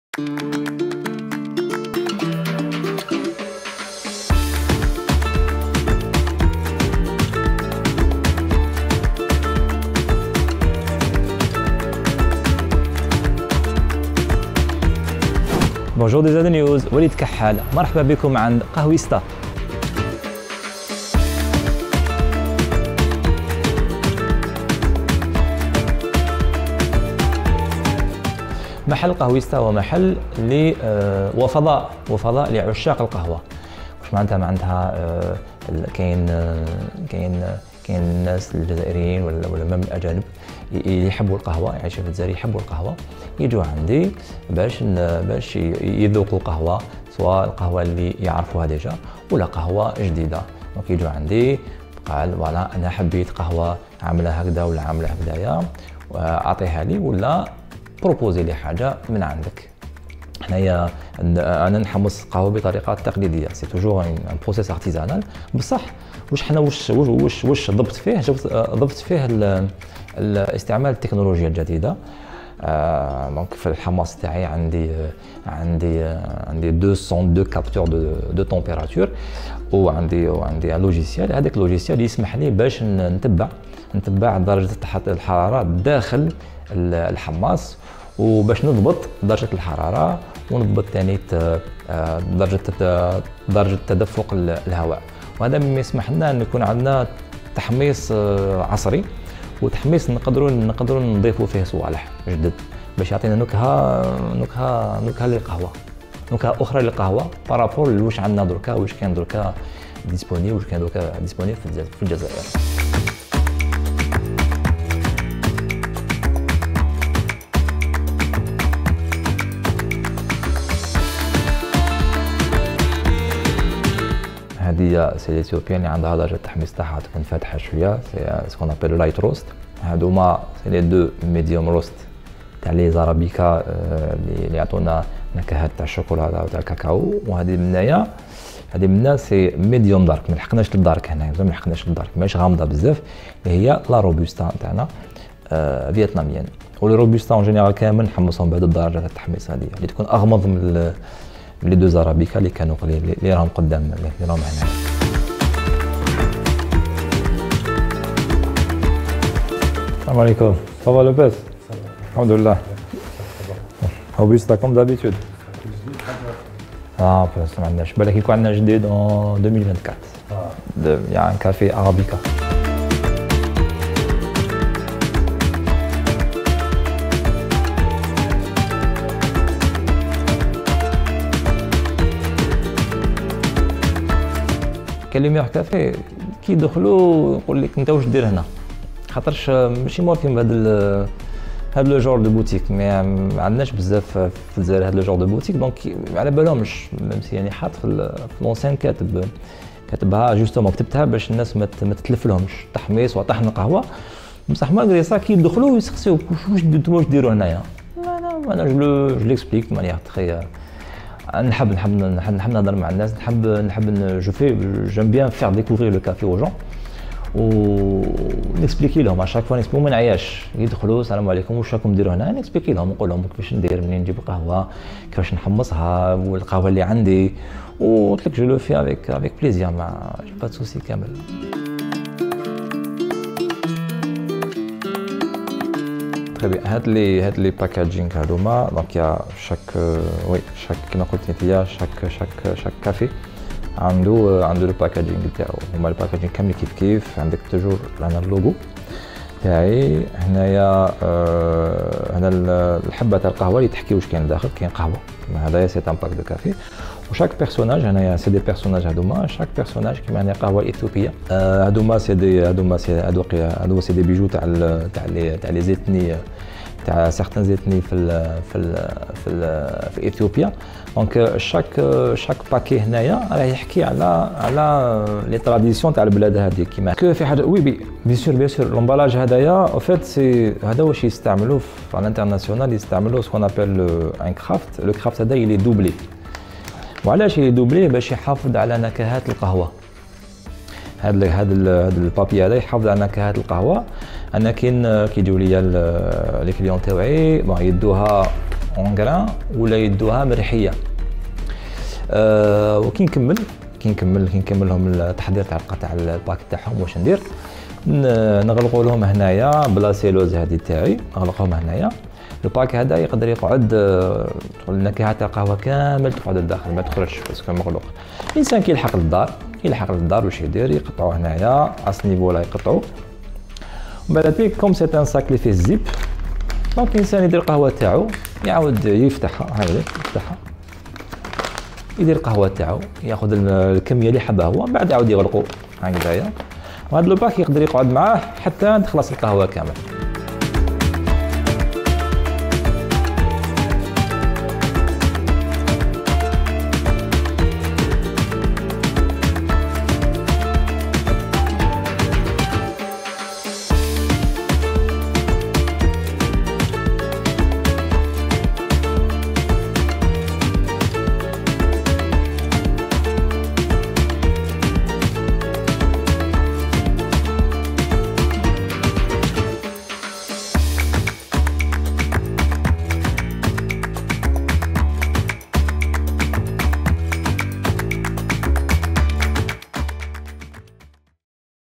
الفكرة الأولى من نوعها في الجزائر..محل "قهويستا" يوفر مختلف أنواع القهوة العالمية للزبائن بطرق عصرية
عشــــاق القهوة...☕الفكرة الأولى من نوعها في الجزائر. محل "قهويستا" يوفر مختلف أنواع القهوة العالمية للزبائن وبطرق عصرية.. 😍